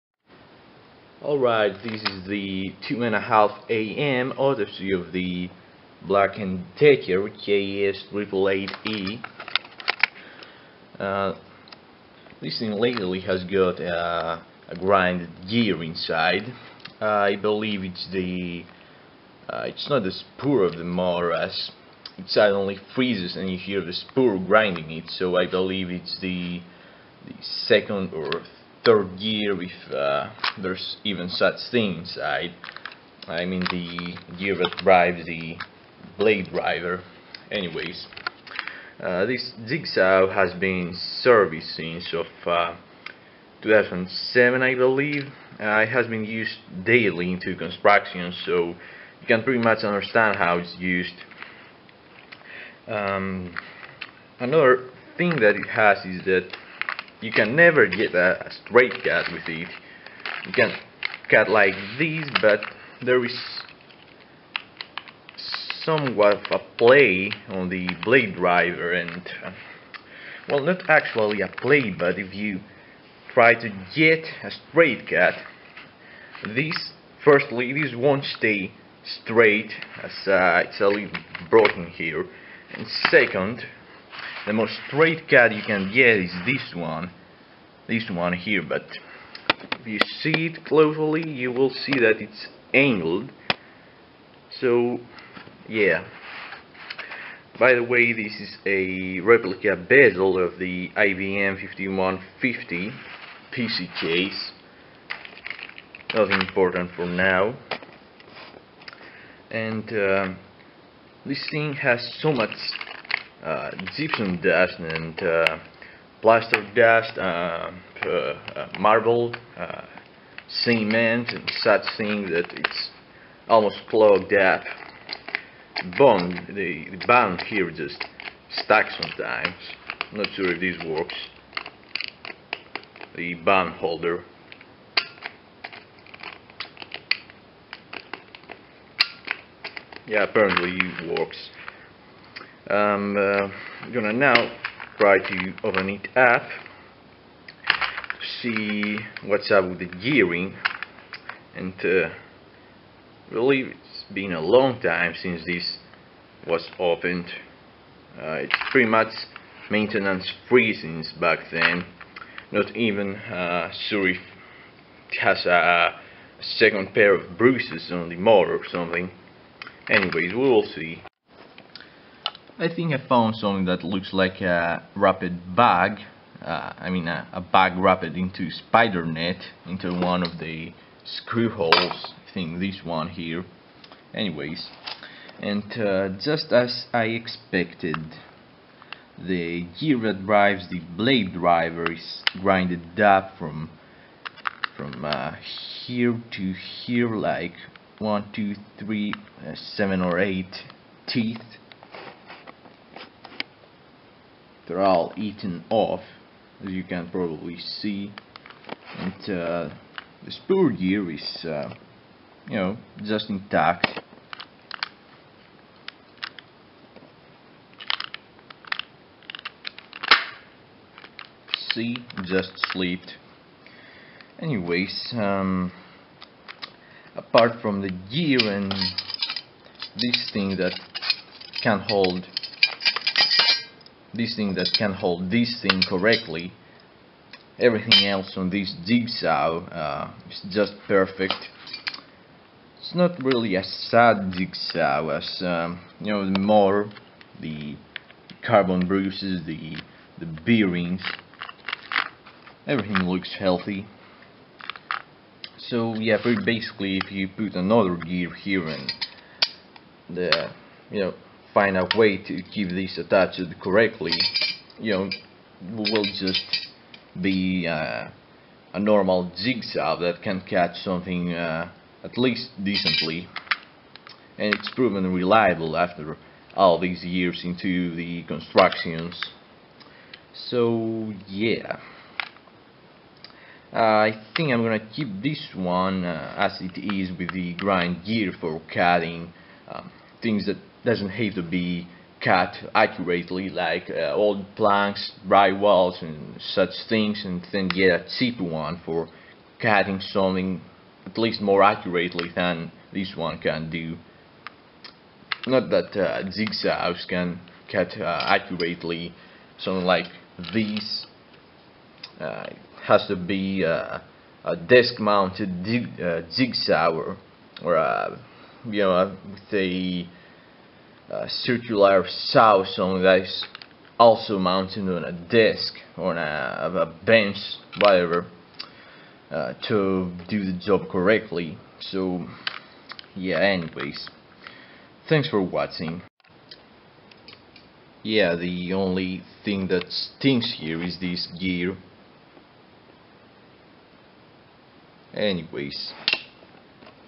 Autopsy Black & Decker Jig Saw Gear Grinding
Autopsying at 2:30AM the old badly used Black and Decker Jig Saw that has Grind Gears and broken base.

This has been used daily in constructions since 2007 or so.